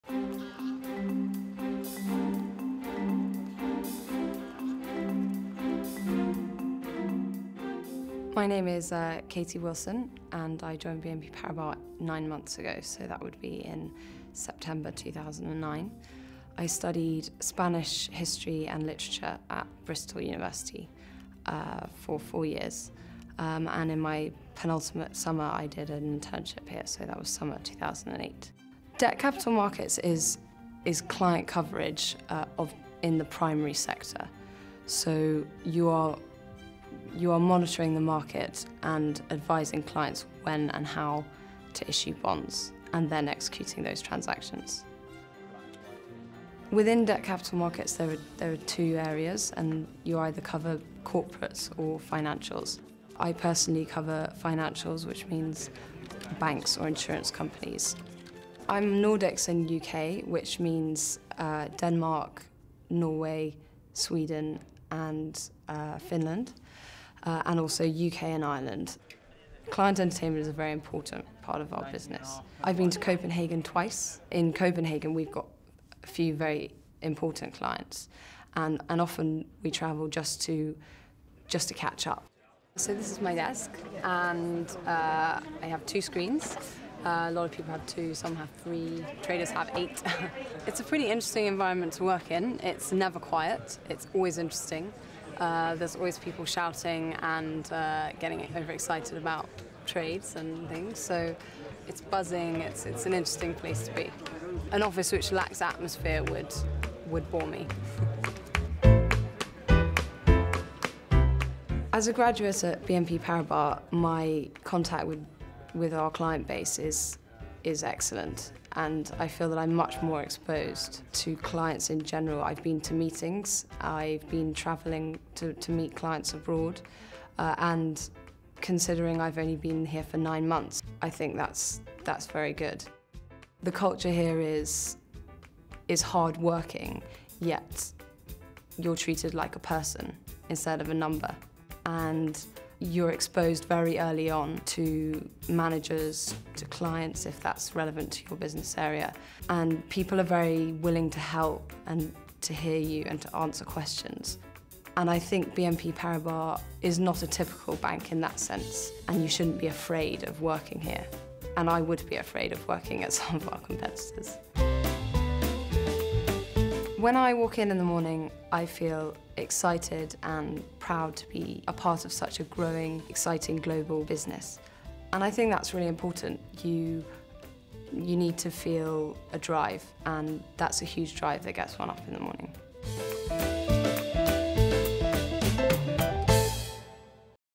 Katie, Fixed Income Debt Capital Markets, BNP Paribas CIB, London
Within Dept Capital Markets there are two areas and you either cover corporates or financials. I personally cover financials, which means banks or insurance companies